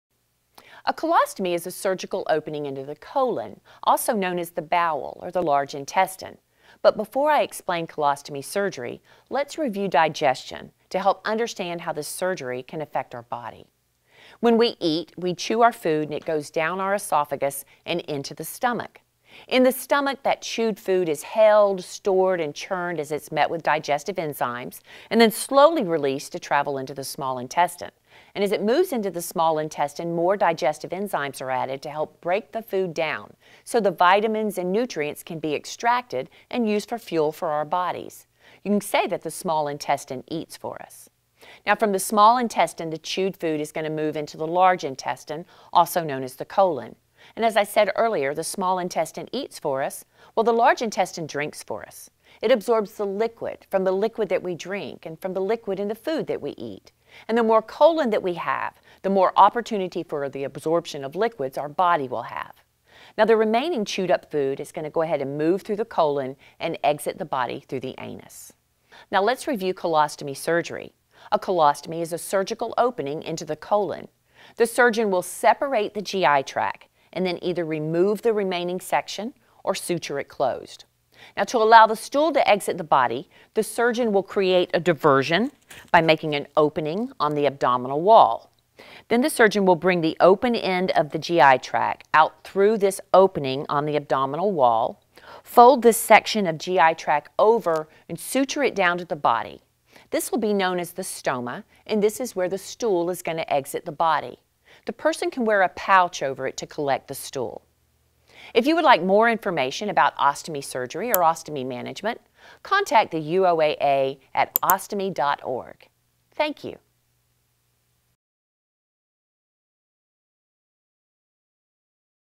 What is a Colostomy?
Colostomy surgery explanation from ostomy nurse Joy Hooper, an Independent expert for United Ostomy Associations of America. Learn more about all things ostomy– including support, advocacy and education